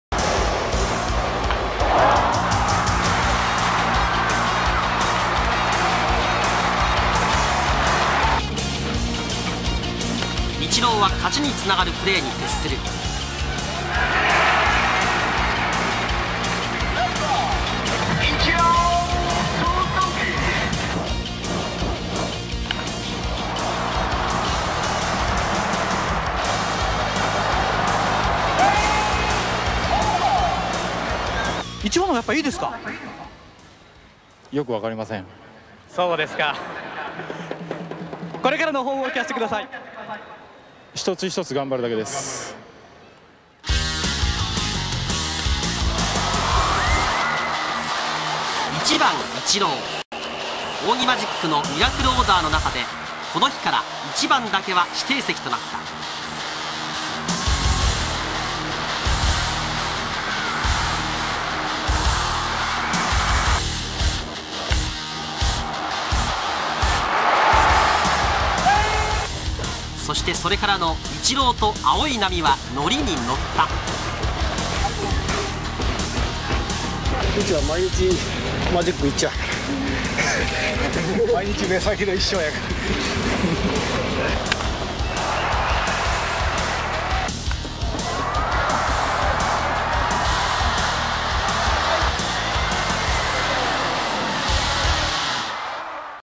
【プレー集】高卒４年目とは思えないイチローの最高レベルの走攻守プレー
高卒４年目とは思えないイチローの走攻守プレー集。
仰木マジックの申し子。高卒４年目でチームの中心選手。